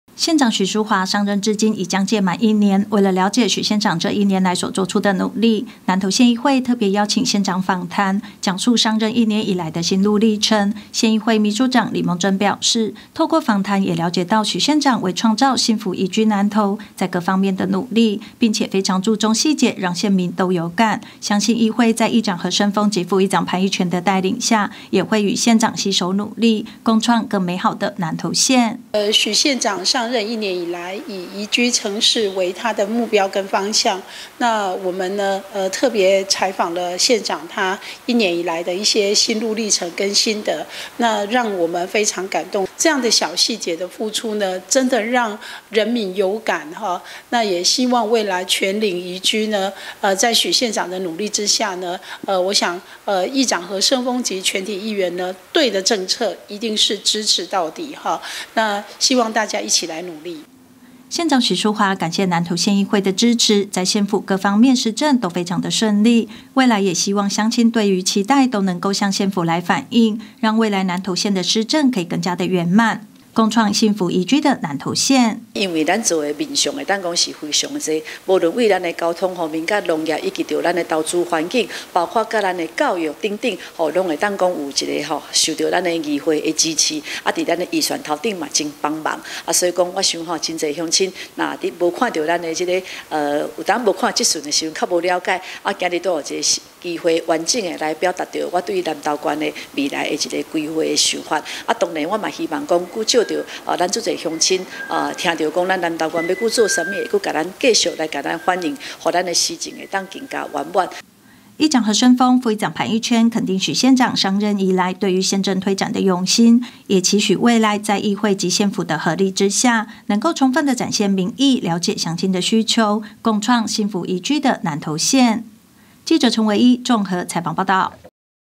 20231223 南投縣議會 民議新聞 許淑華縣長上任一年心路歷程 盼與議會及縣民共創幸福宜居南投
縣長許淑華上任將屆滿一年，為了瞭解許縣長這一年來所做出的努力，南投縣議會特別邀請縣長訪談，講述上任一年來的心路歷程，議長何勝豐、副議長潘一全肯定許縣長上任以來，對於縣政推展的用心，也期許未來在議會及縣府的合作之下，能夠充分展現民意，了解鄉親的需求，共創幸福宜居的南投縣。